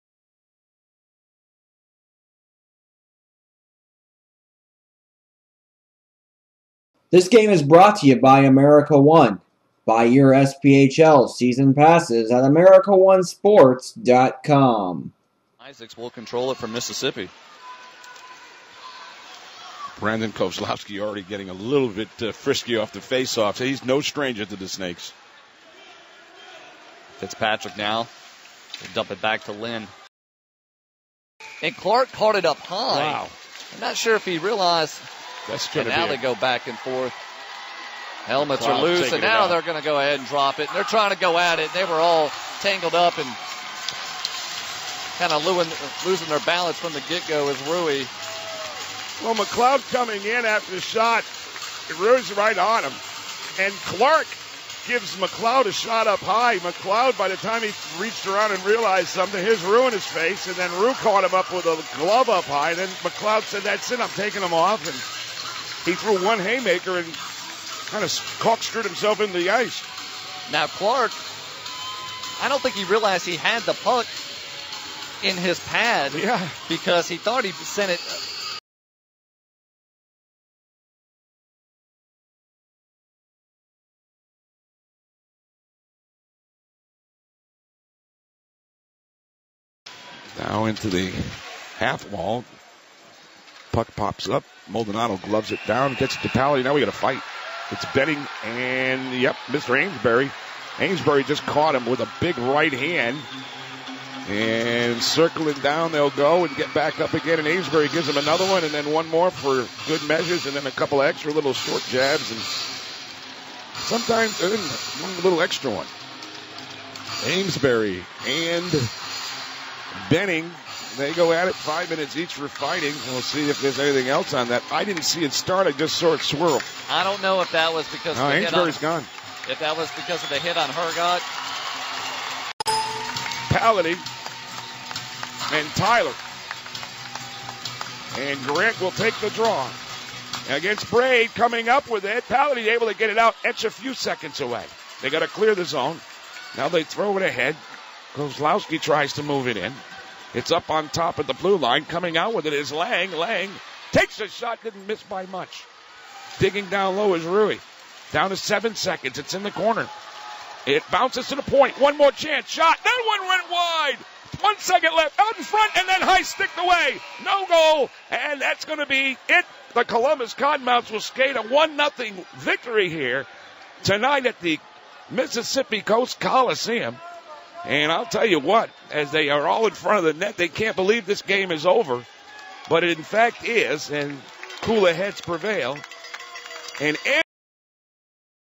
11-21-12 Mississippi Surge vs. Columbus Cottonmouths America One Game Highlights
The Game Highlights by America One of the November 21st, 2012 SPHL hockey game between the Mississippi Surge and the Columbus Cottonmouths. The Cottonmouths were the victors 1-0.